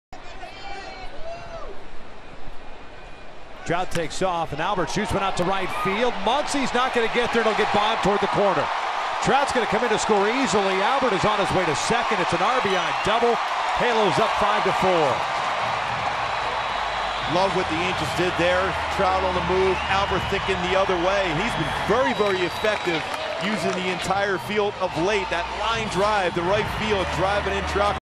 OAK@LAA: Pujols plates Trout and surpasses Gehrig
Albert Pujols belts a double to right to score Mike Trout, passing Lou Gehrig for ninth on the all-time extra-base hits list